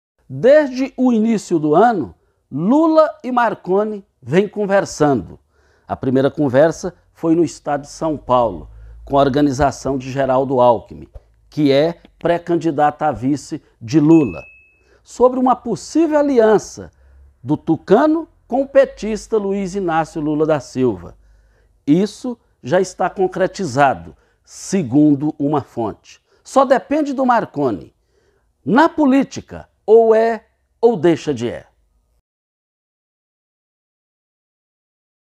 Lula e Marconi juntos no mesmo palanque em Goiás
Desde o início do ano Lula e Marconi vem conversando, o assunto seria sobre uma possível aliança do tucano com o petista, segundo uma fonte a decisão está nas mãos de Marconi Perillo.